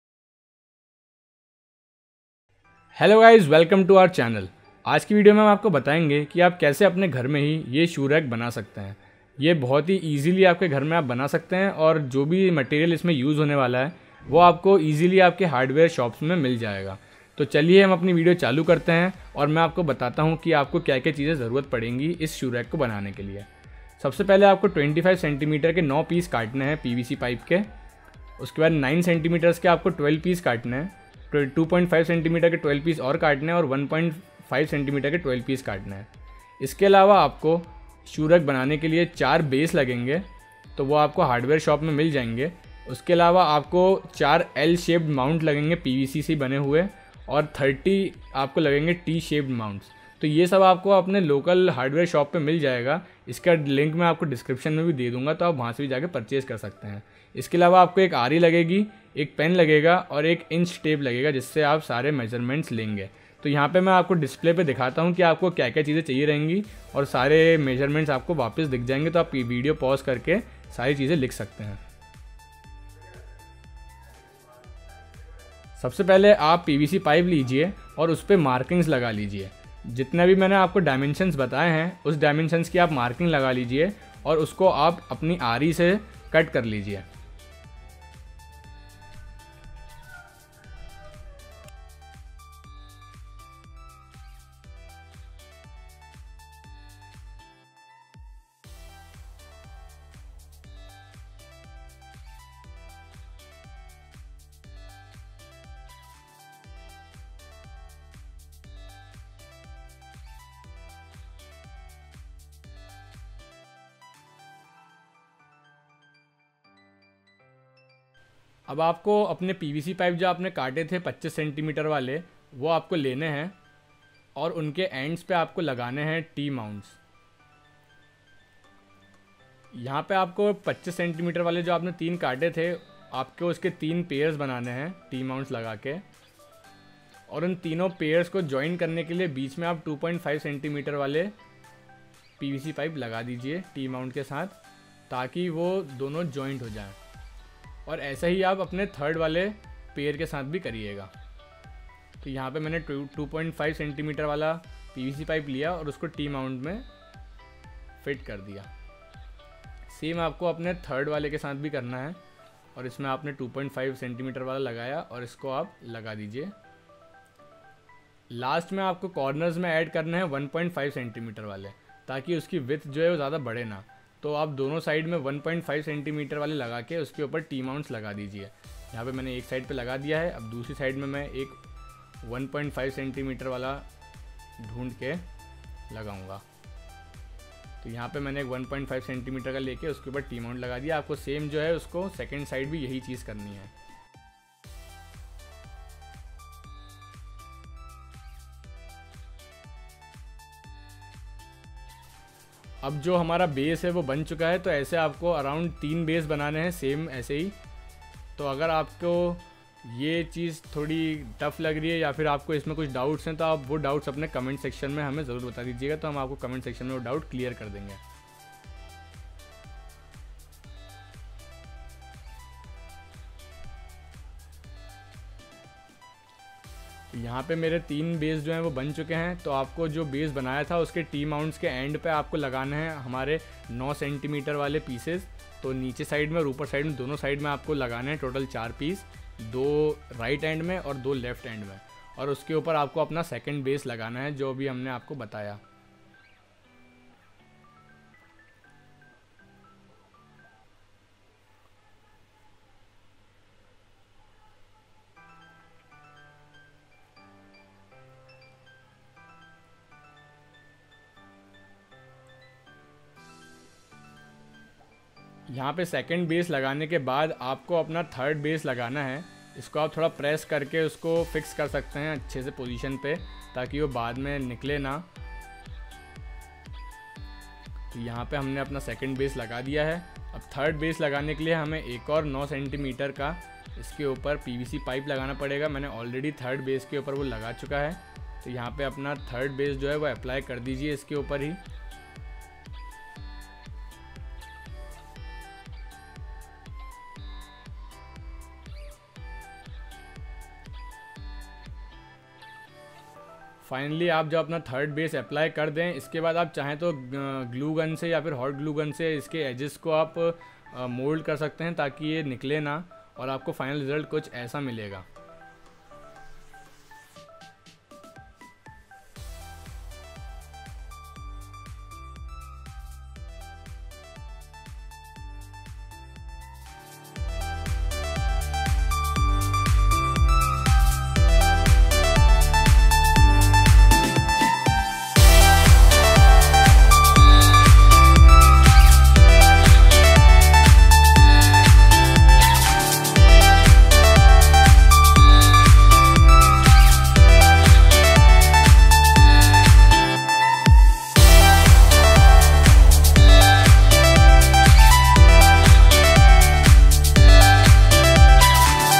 DIY Make Shoe Rack from PVC Pipes at Home Easily - Art and Craft Hindi Tutorial
This Video Demonstrate How to Make Shoe Rack at Home Easily with PVC Pipes

Items Required :-
25 cm x 9 Pieces
9 cm x 12 Pieces
2.5 cm x 12 Pieces
1.5 cm x 12 Pieces
L Shaped Mount x 4
T Shaped Mount x 30